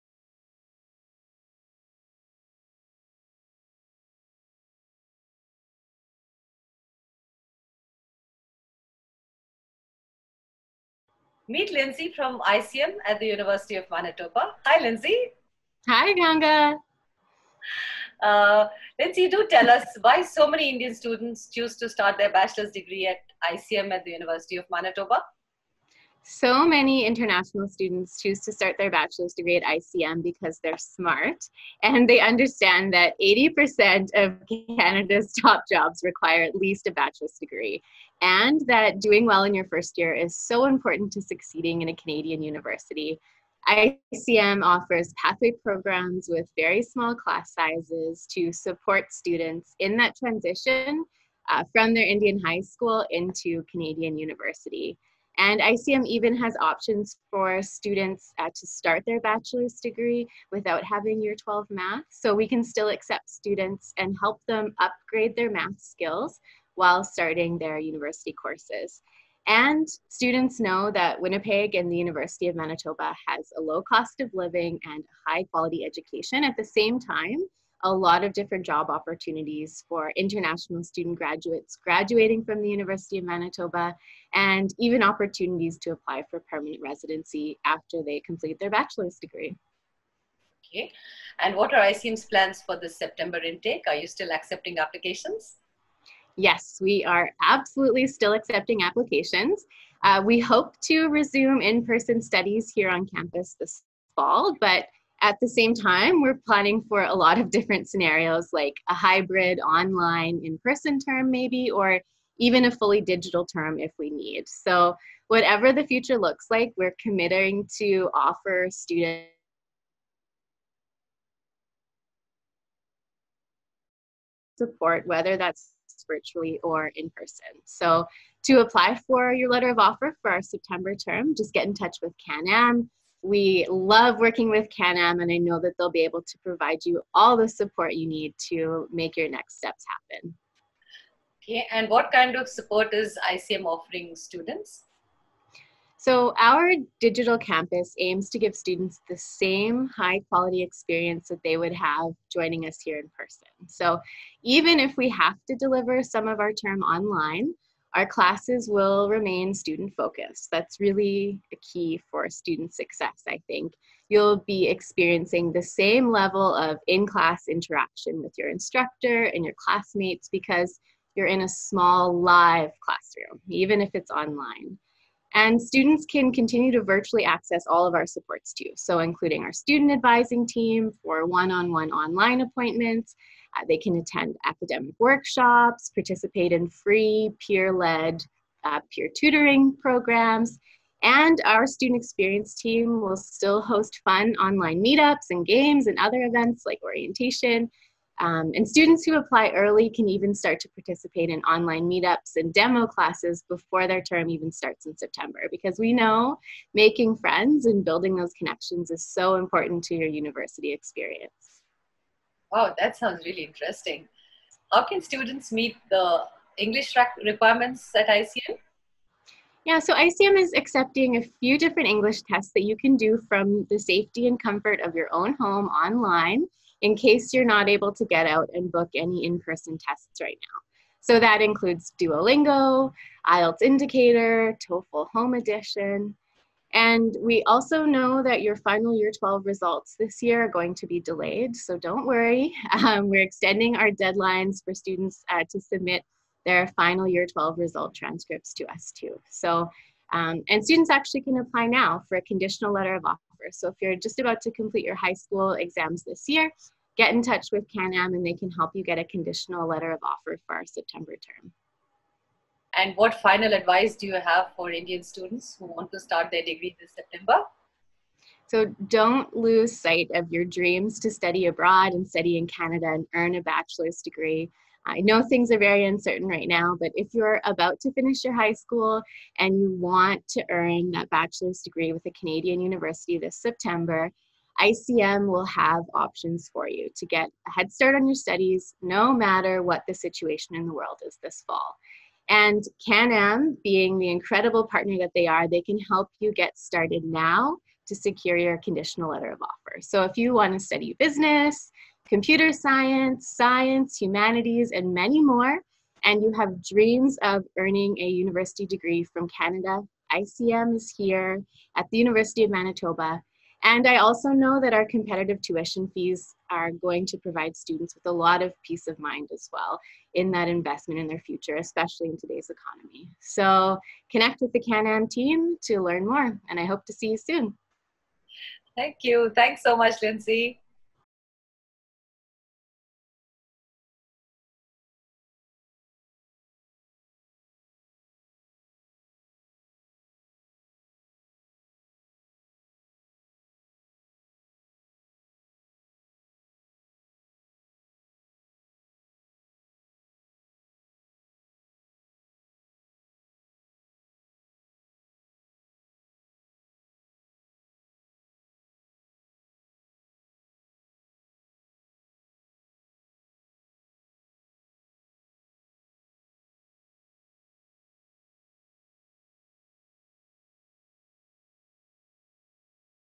ICM @ University of Manitoba:
*Why so many Indian students choose to start their bachelor’s degree at ICM at the University of Manitoba
*ICM still accepting applications for the September intake
*Options available for no maths in yr12
*Meeting ICM’s English requirements
*Support ICM is offering to students
*Advice for Indian students who want to start their degree this September. @icmanitoba

To know more view video and call Team Canam at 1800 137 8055.